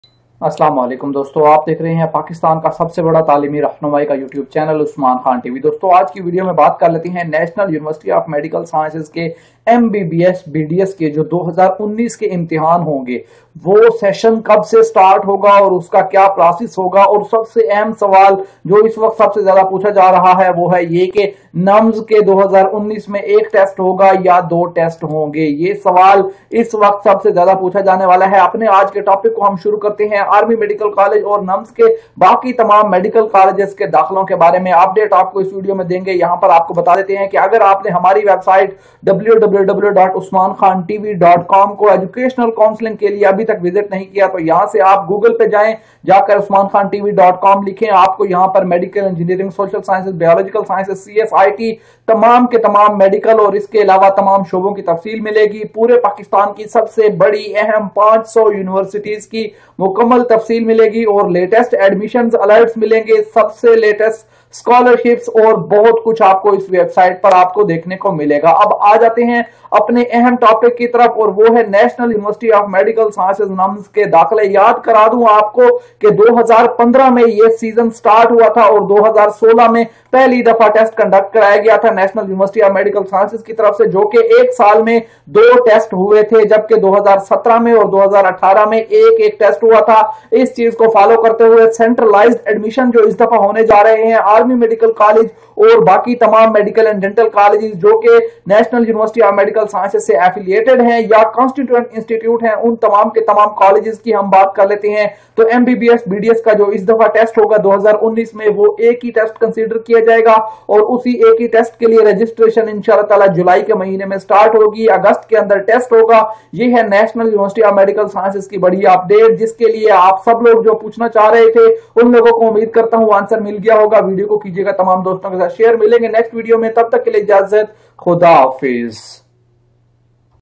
How Many Test NUMS will Conduct in 2019 !! MBBS/BDS 2019-20 Admissions
In This video i will tell you how many test nums will conduct in 2019 for the mbbs and bds admissions session of 2020 .